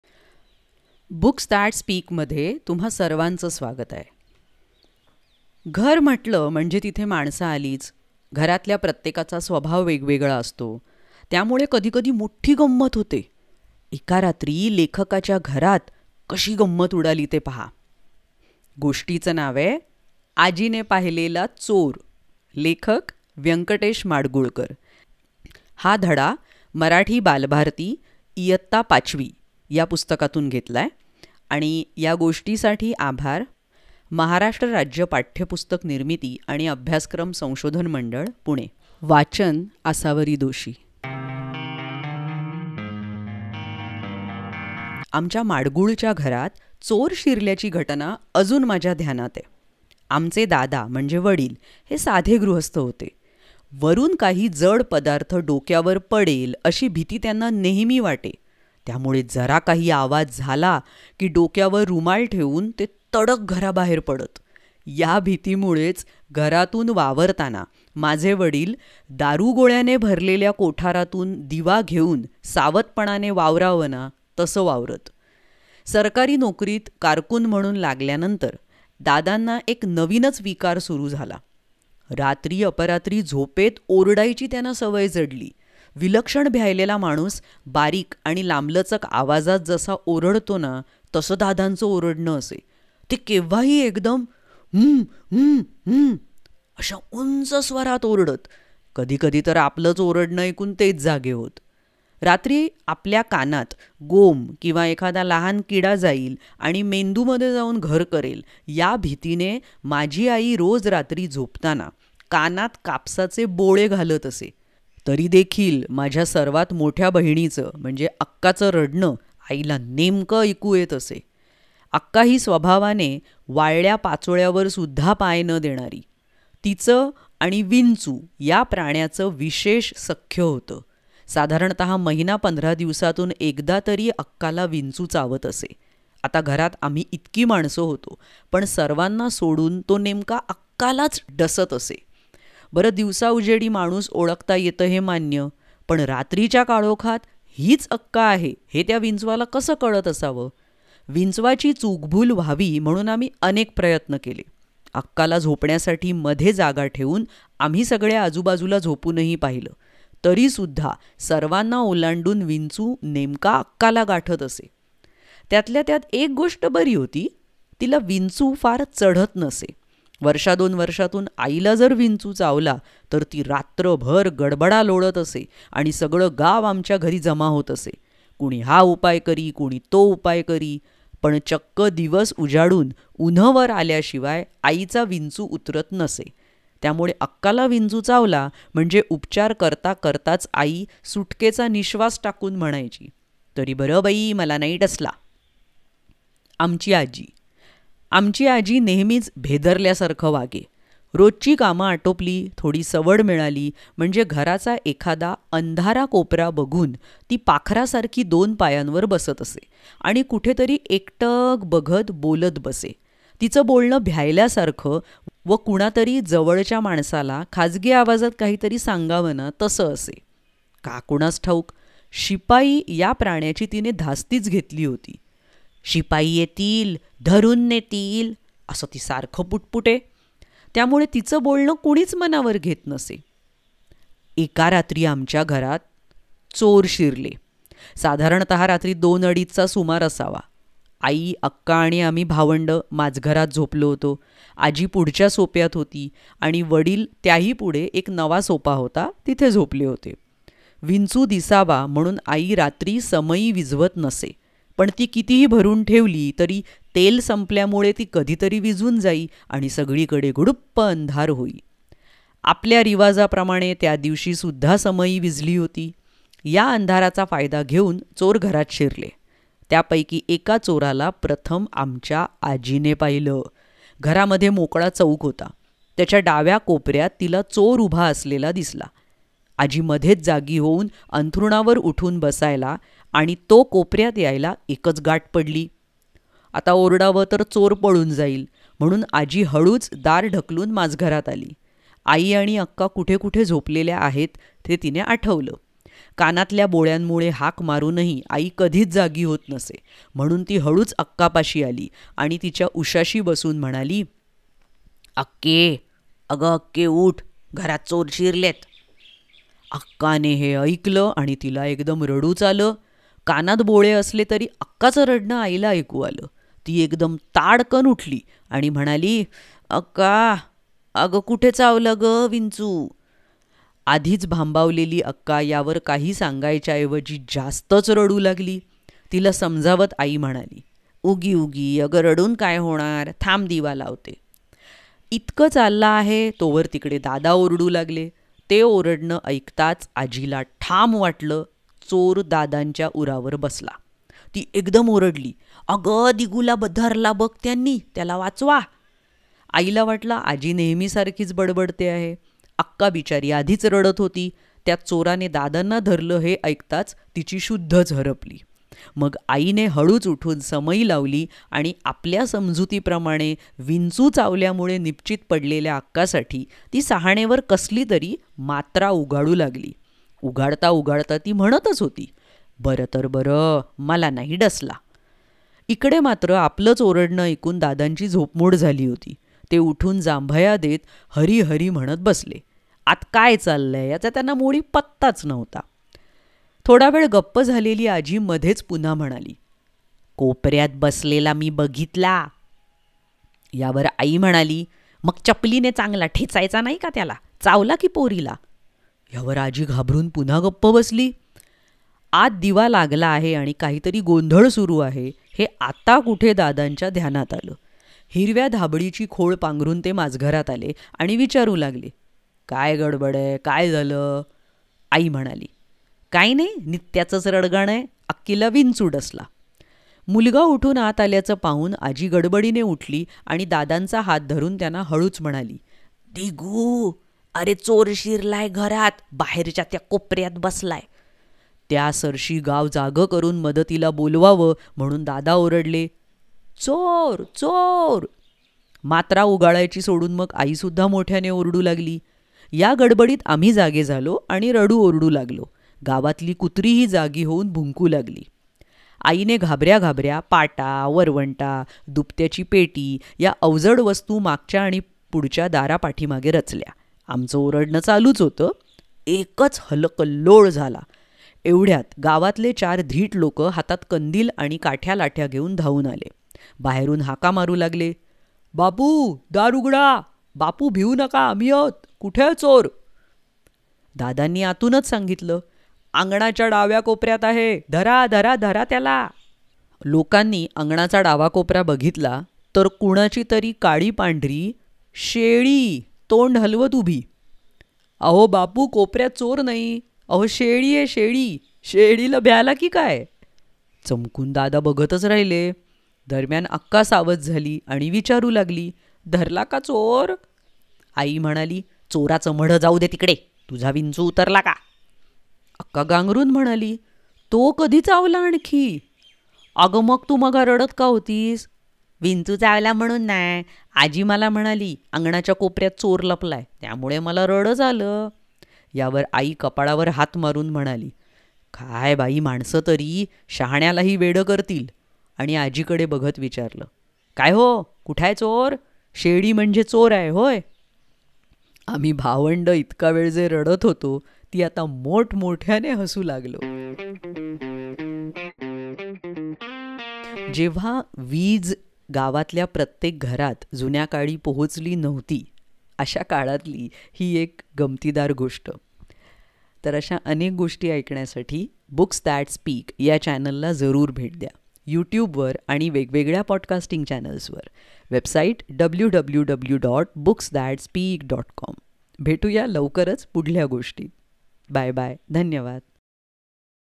आजीने पाहिलेला चोर (My Grandma saw a Thief) - Marathi Stories for Kids
When there is a home, there are different personalities. Everyone has their own peculiar nature, own style. And that creates an fun-filled environment.
One day, a thief entered the house and then.
Let's enjoy what happens next in this story from Marathi Balbharti Std. 5 (1984 edition).
Written by Vyankatesh Madgulkar.

Thanks to Maharashtra State Bureau of Textbook Production & Curriculum Research, Pune.

To receive updates about Online and Offline storytelling events from Books That Speak, join the whatsapp group: